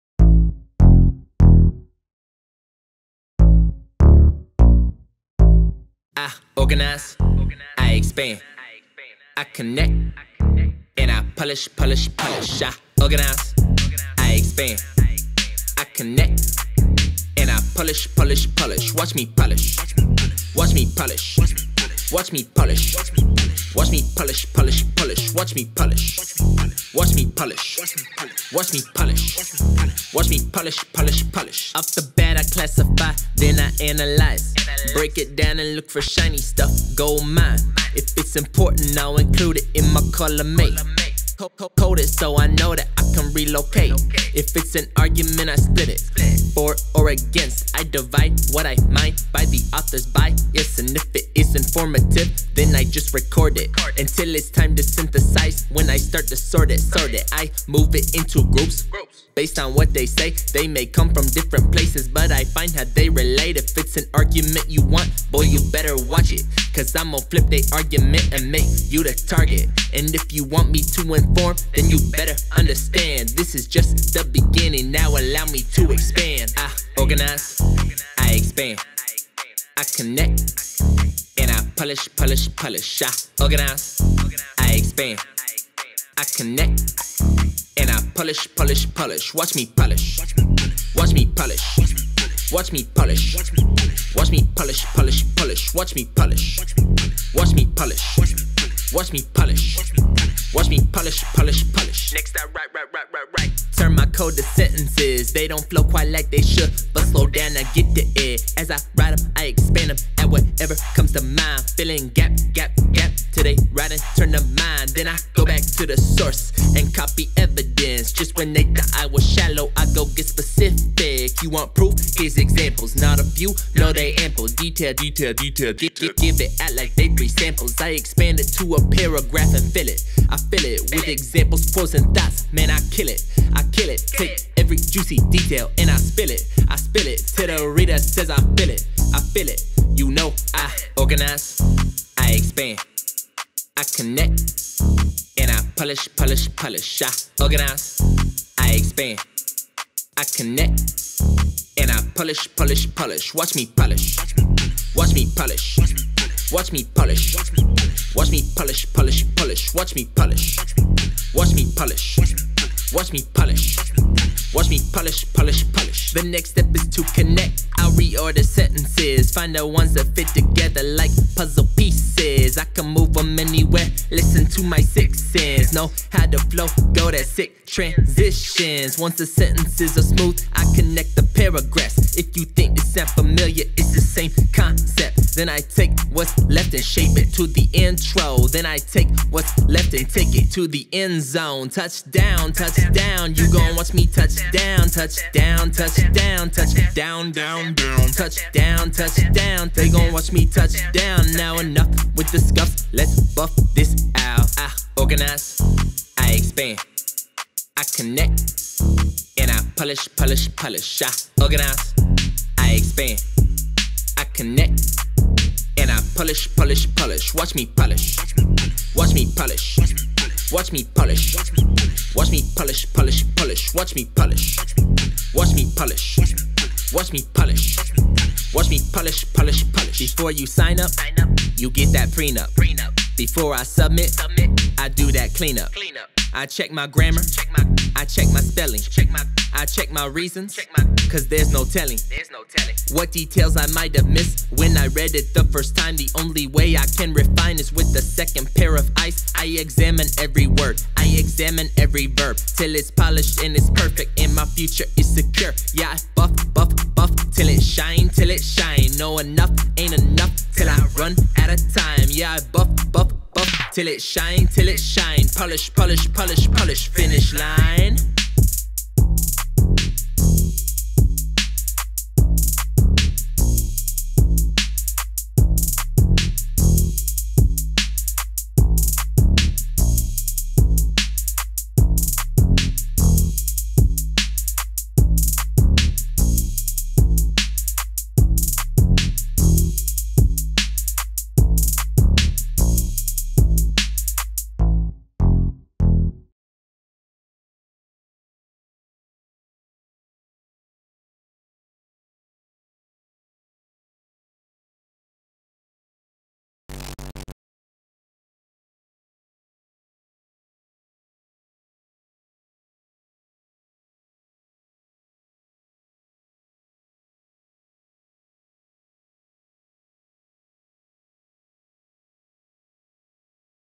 WATCH ME POLISH (How to Write, Refine, and Polish an Essay for Publication)
Writing an essay may never be easy, but it can be stress-free, fun, and rewarding.  Introducing "Watch Me Polish," the new, irresistable approach to writing essays.